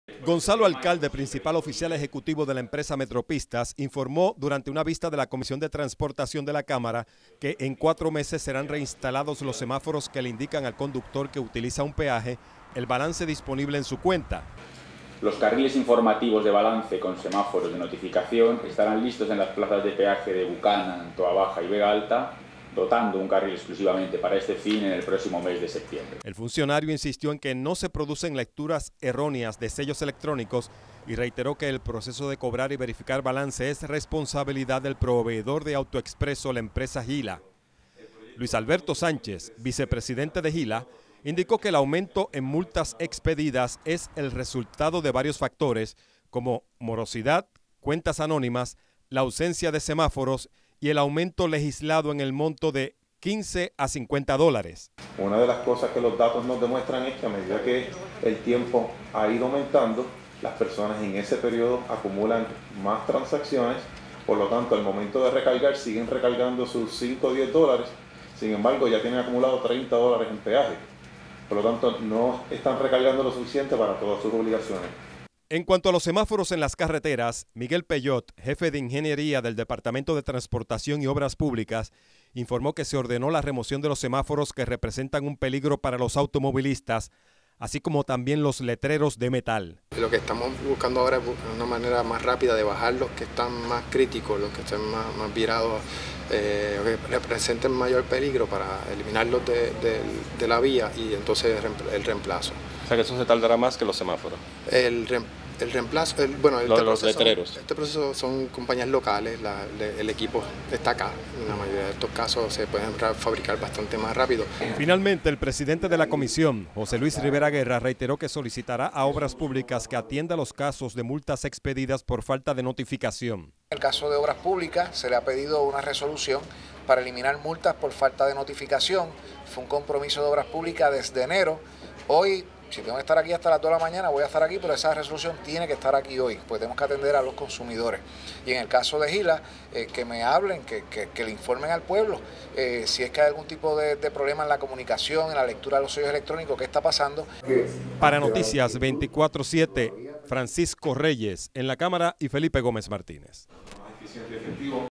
Justifican causas del aumento en multas de AutoExpreso
Los semáforos que le indican al automovilista el balance disponible al pasar un peaje serán reinstalado próximamente; mientras, la legislatura presiona a DTOP para que reevalúe multas acumuladas por falta de notificación. Felipe Gómez amplía.